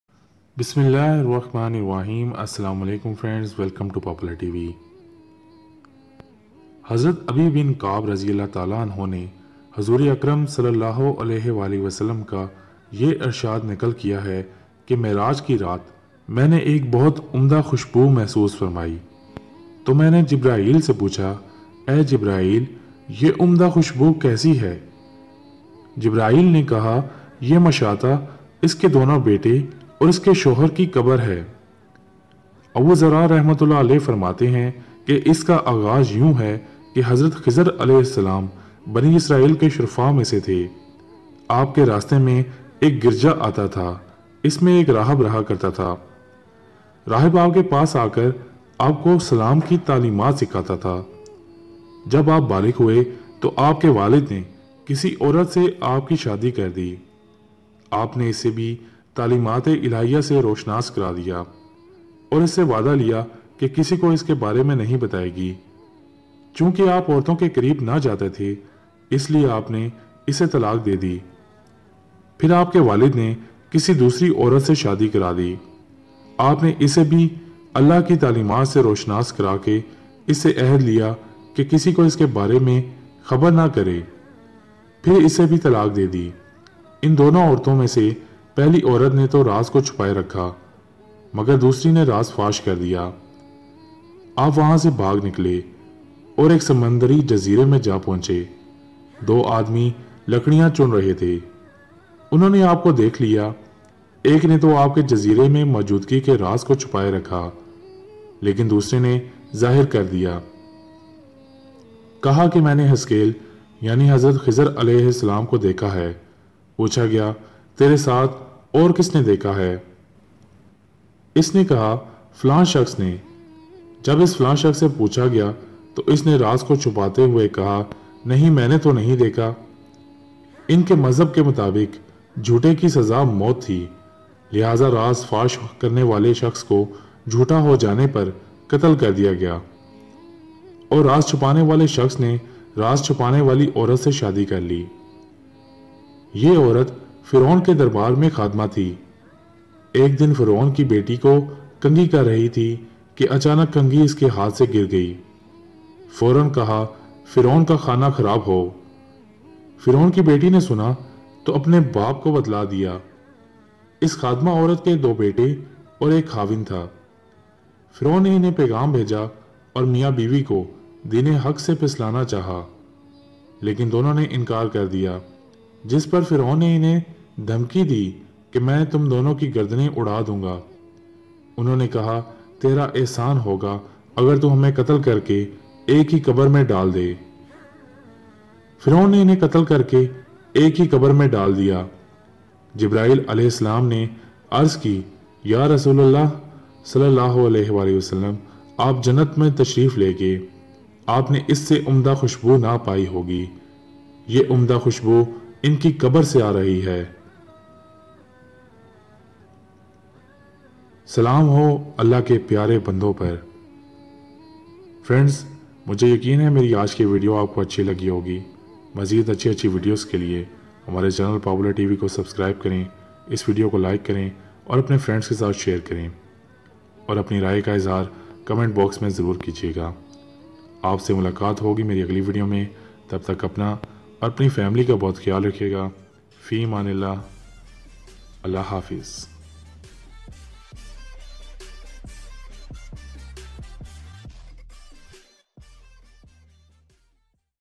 Aese Qabar Jis Ki Khushbu Jannat Mae Mehsos Ki | Nabi Kareem SAWW | Jannat ki Khushboo
Welcome To Popular TV

Is Video Mae Hazrat Khizar AS K Waqia, Ka Zikar kiya gya ha aur Nabi Pak SAWW Nay Aik Aesay Khandan Ki Khusboo Jannat mae Mehsos ki Jis ko Firon Nay Aik He Qabar mae Qatal Kar K daal diya tha Nabi Kareem SAWW nay Khushboo Jannat Mae Mehsos ki to Jibrael AS Nay Btaya k yae uos khandan ki khushbu hai jo aik e qabar mae haen
VOICE :
( Usama Saif Uallah Owner of Popular TV Channel And A Voice Over Artist )
Watch Hazrat Lal Shahbaz Qalandar RA ki Karamat Ka Waqia | لعل شہباز قلندر | Full Waqia By Popular TV